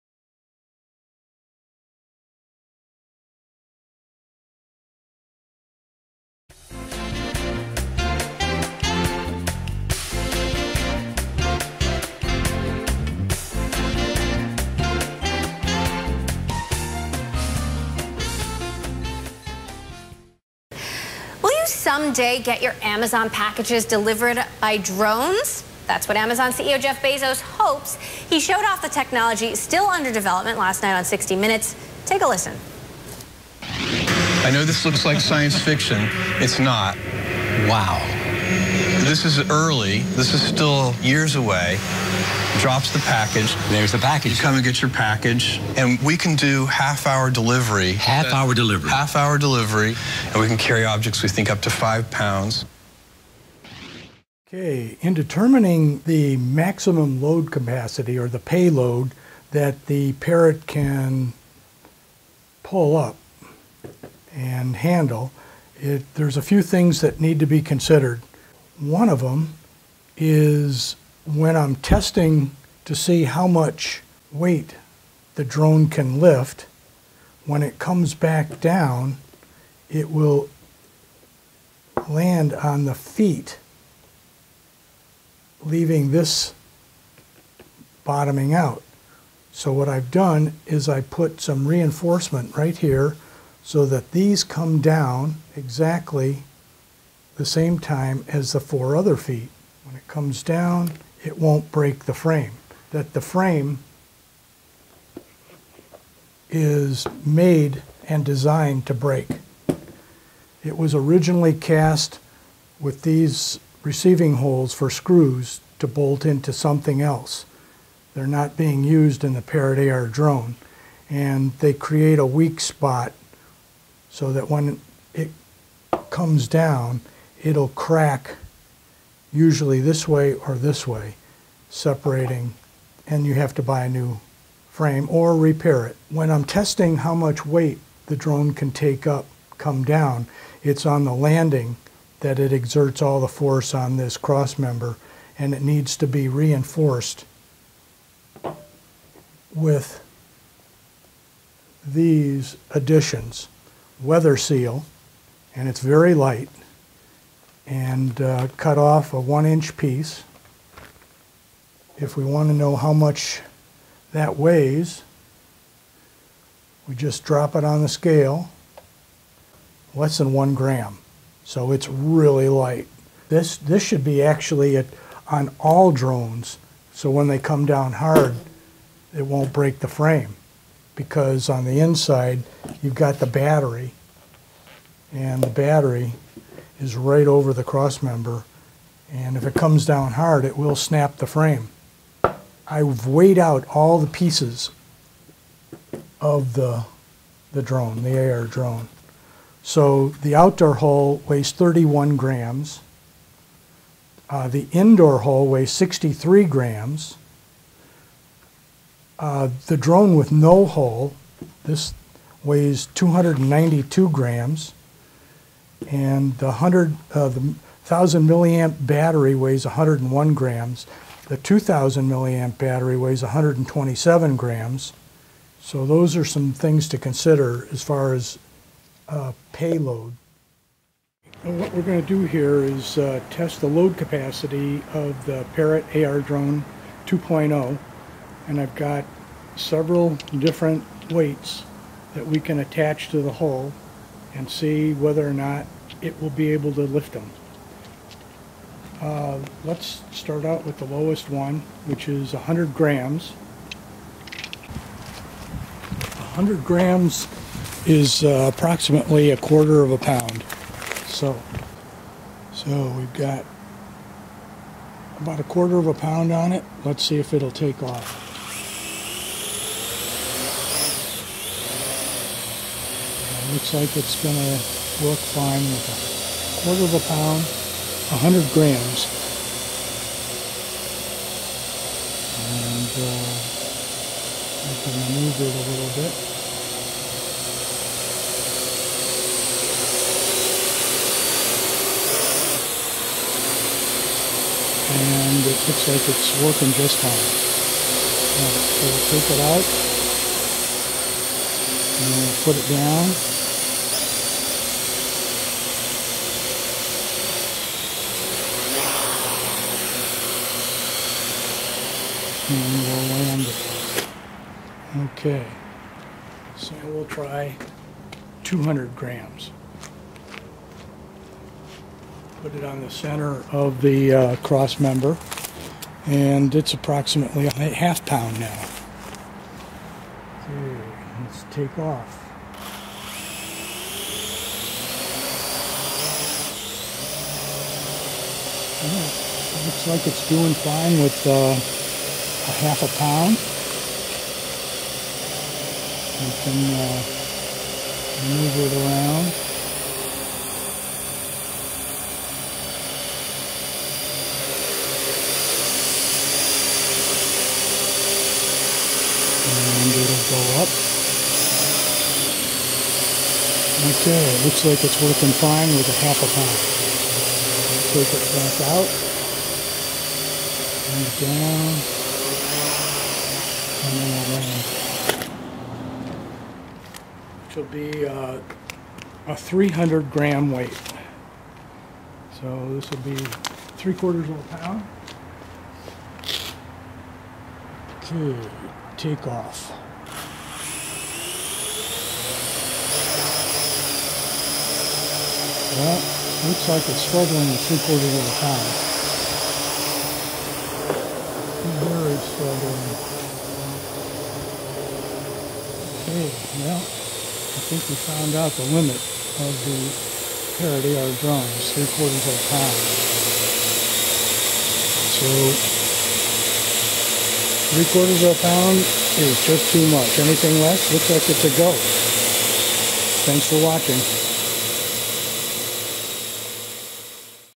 Parrot AR Drone Package Delivery
This video answers the age old question, "What is the maximum load capacity of the Parrot AR Drone 2.0."  Amazon is contemplating using helicopter-drones for package delivery if they can persuade the F.A.A. to alter the rules of the airways to allow drone commercial flights.